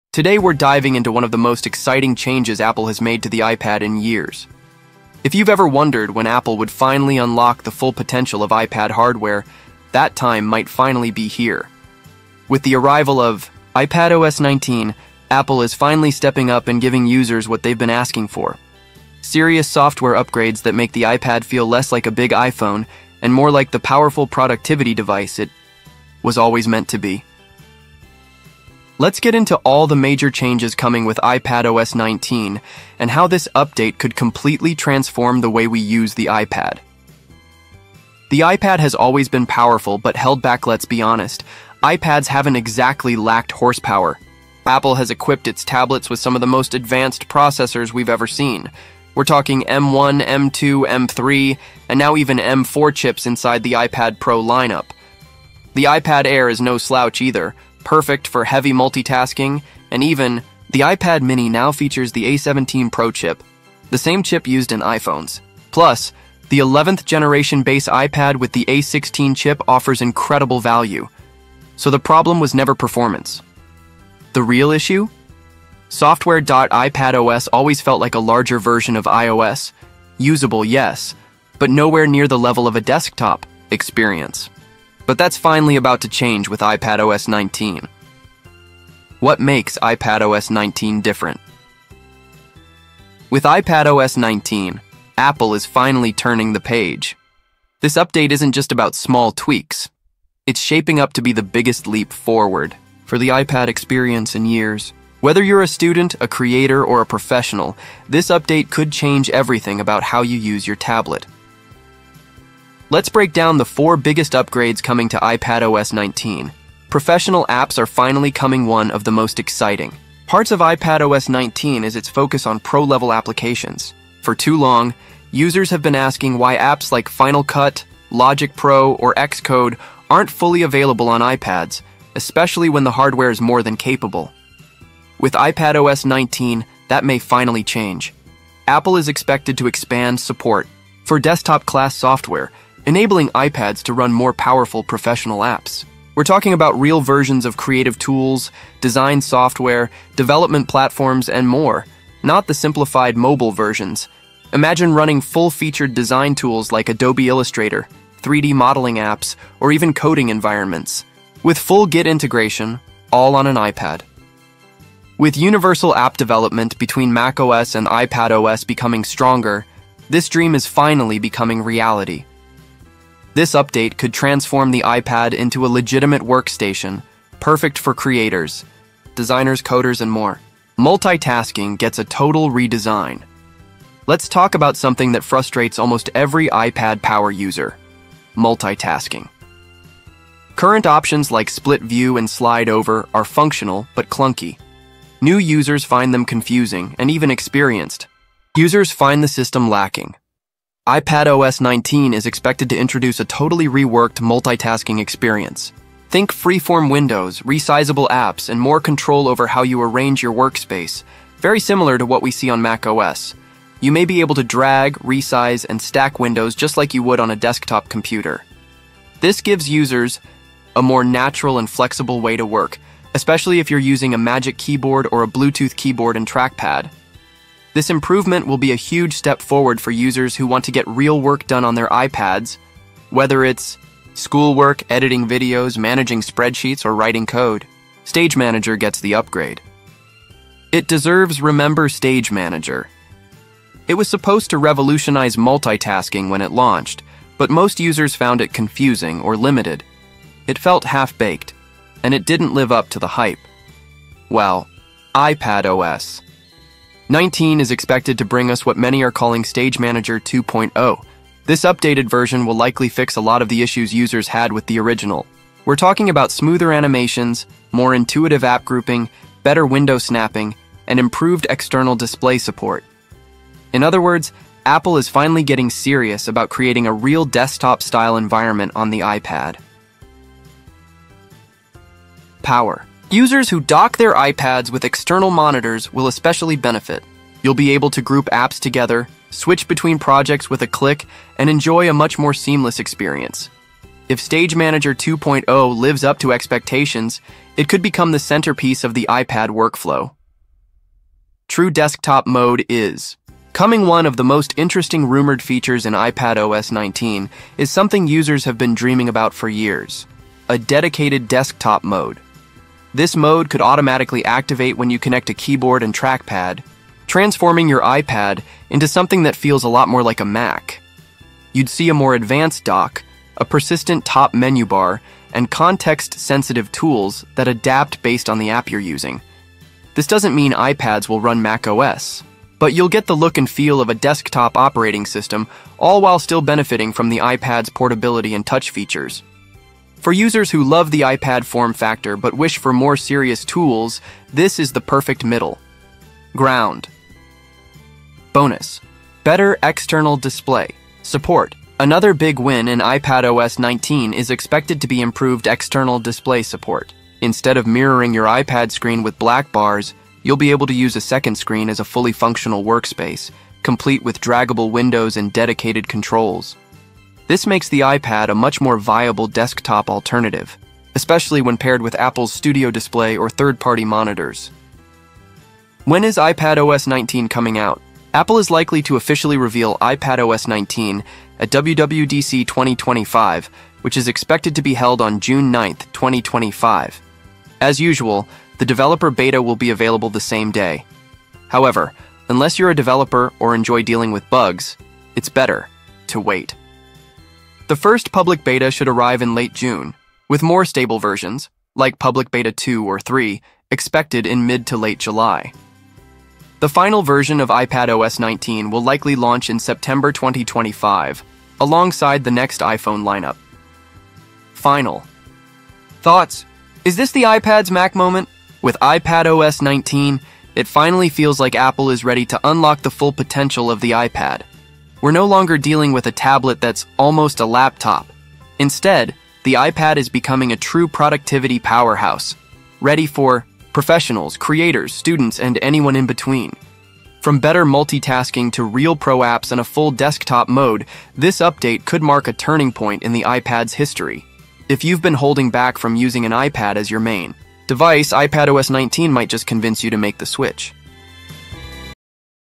iPadOS 19 Is FINALLY Getting What We Wanted - MacOS DESIGN Confirmed!
The wait is finally over! iPadOS 19 is set to bring the features we've all been asking for, and yes — a MacOS-style redesign is officially happening! In this video, we dive deep into the confirmed changes, expected UI updates, and the brand-new functionality that will take your iPad experience to a whole new level. Apple is finally making the iPad feel more like a true Mac — multitasking, windowed apps, advanced productivity tools, and more are on the way.

🔥 Discover what’s new in iPadOS 19
🖥️ Explore the MacOS-style redesign
📱 See how your iPad is becoming more powerful than ever
🚀 Performance, features, and developer insights

If you've ever wanted your iPad to feel like a MacBook, this update might be the one that changes everything!